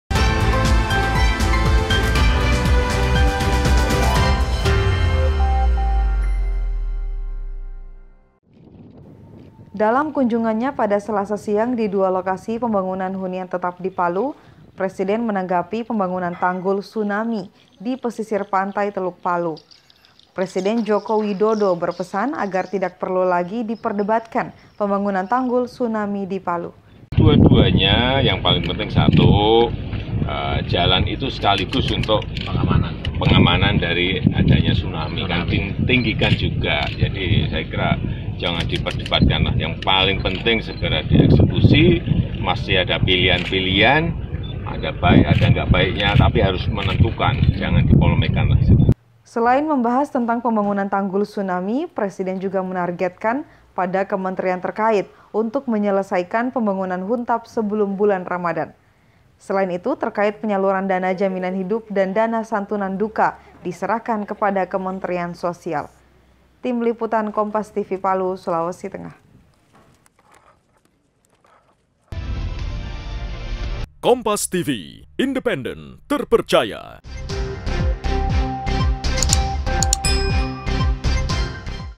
TANGGAPAN JOKOWI TERKAIT TANGGUL TSUNAMI DI PALU,KOMPASTV PALU
PRESIDEN JOKO WIDODO MENANGGAPI PEMBANGUNAN TANGGUL TSUNAMI YANG SEMPAT DIPERDEBATKAN. MENURUTNYA, TANGGUL ITU AKAN TETAP DIBANGUN DAN FUNGSINYA AKAN MEREDAM JIKA KEMBALI TERJADI BENCANA TSUNAMI DI PESISIR PANTAI TELUK PALU.